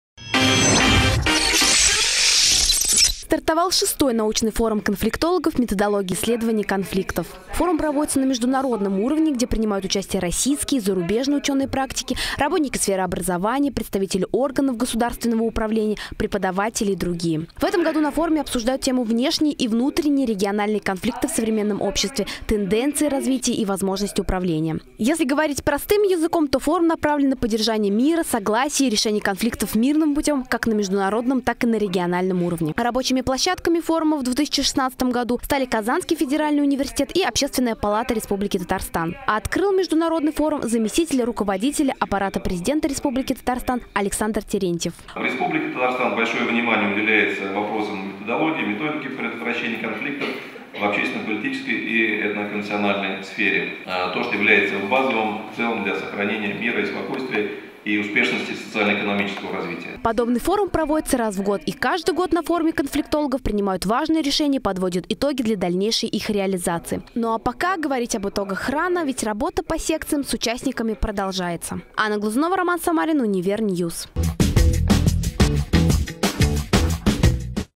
КАК РЕШИТЬ КОНФЛИКТ
КФУ на VI международном форуме конфликтологов собрал: научных сотрудников, работников сферы образования, ученых-практиков.